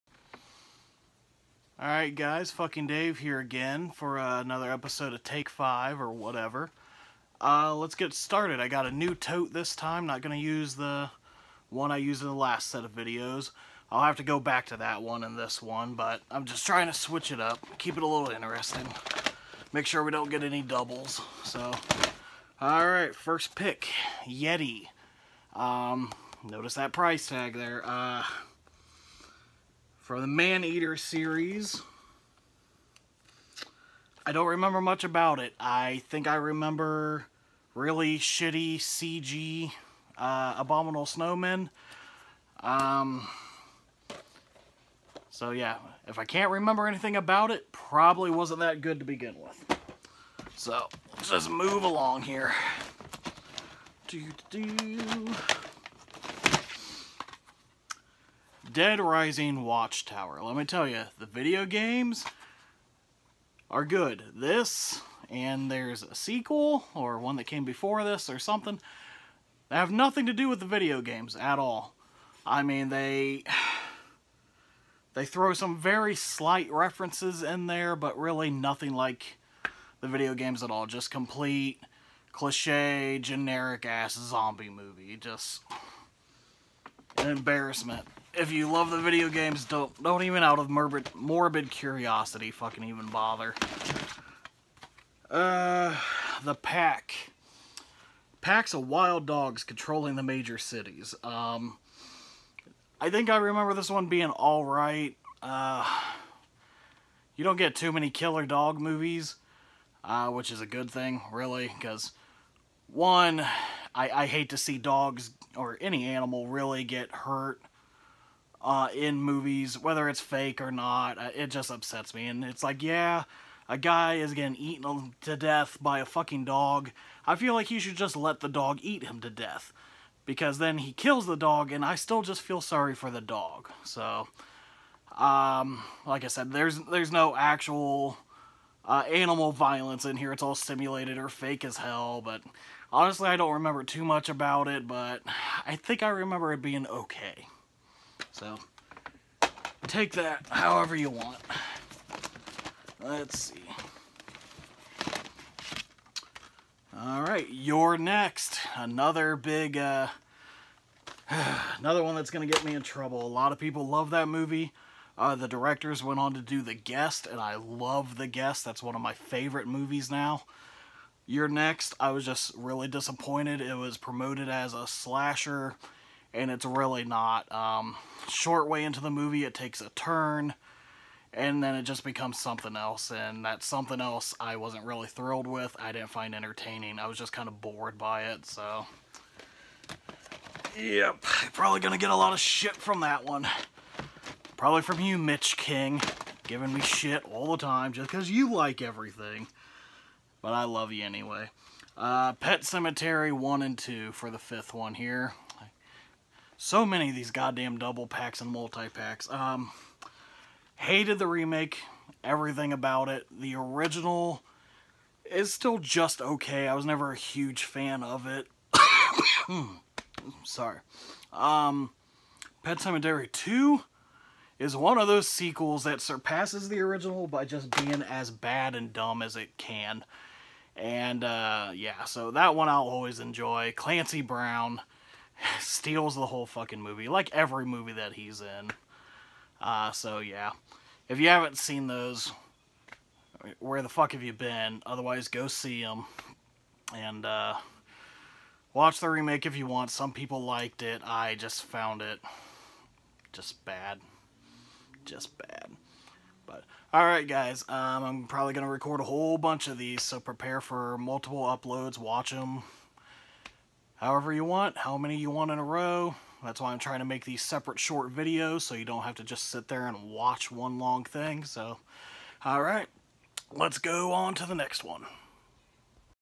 Take Five (8)
Fucking Dave picks five random dvds out of his collection and has five minutes to talk about them.